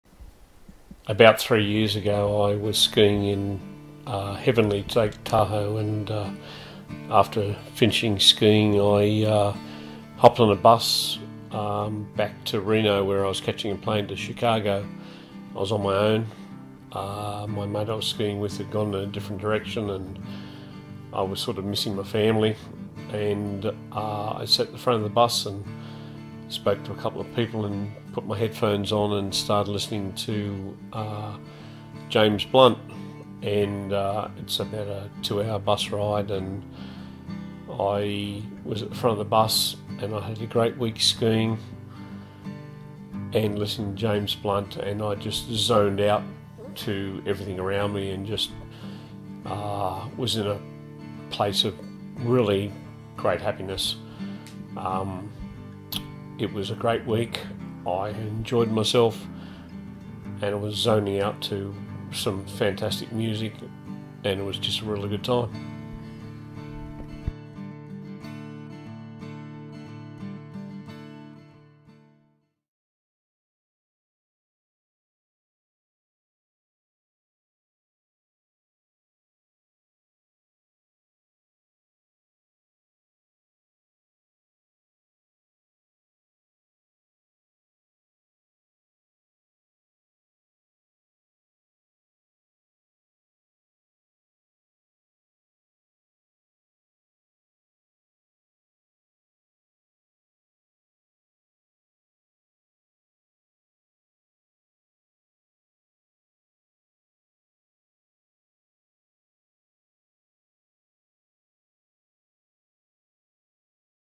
Places for One
A audiovisual story describing feeling and experiences by those present in place or participating in activities that make them feel a sense of happiness and peace